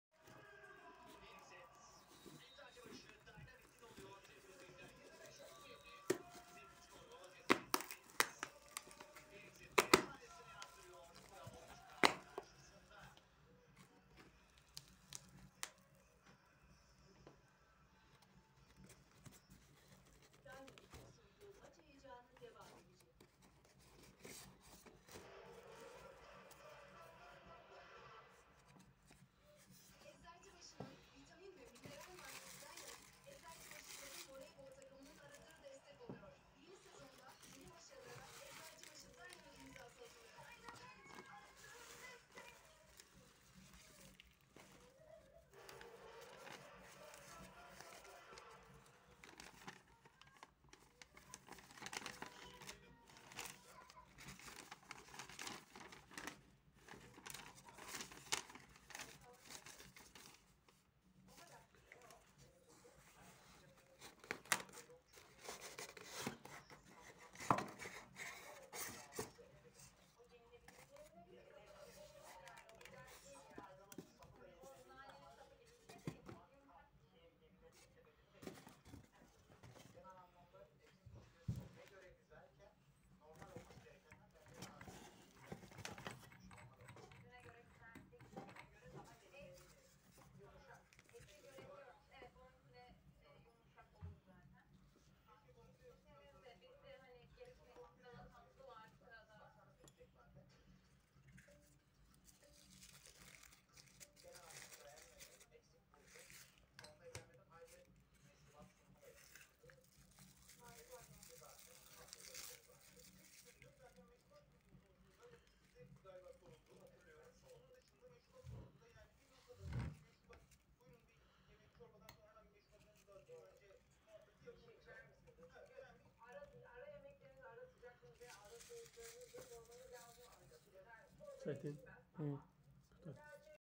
SSRİ-dən qalma işıq mikroskopun kutu açılımı.
Sovetlər zamanımdan qalma işıq mikroskopunun kutu açılımı.Pasportunda istehsal tarixi 22 noyabr 1986 cı ildir